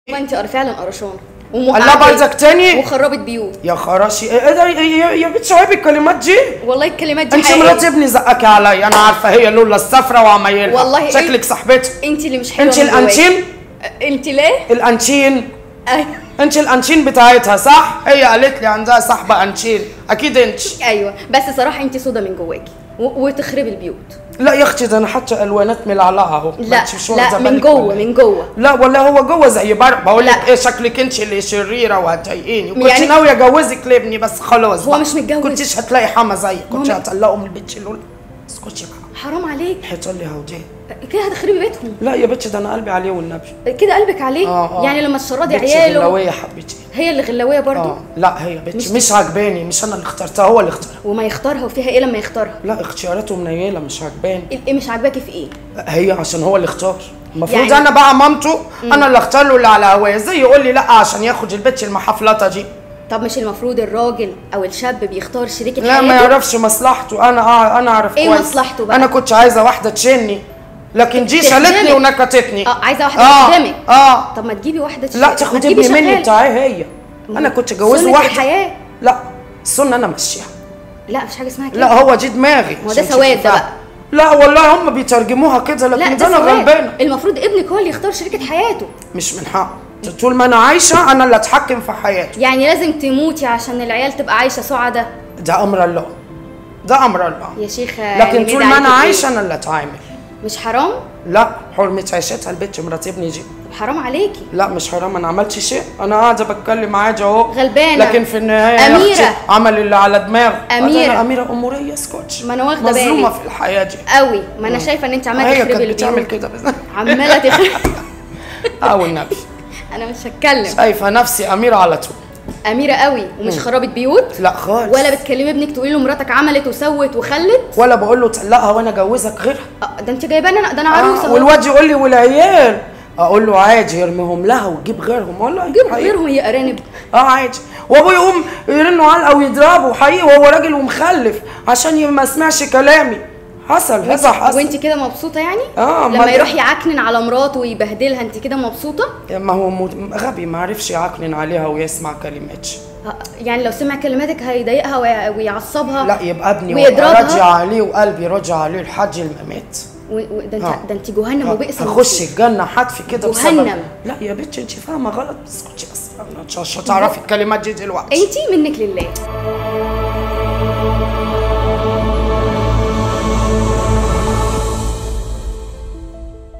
صدق اللي قال الحما عمي - تعالوا نشوف سخنت ابنها إزاي ضد مراته .. تفاصيل كوميدية مع البلوجر بومبا
ـــــــــــــــــــــــــــــــــــــــــــــــ
موقع صدى البلد الإخباري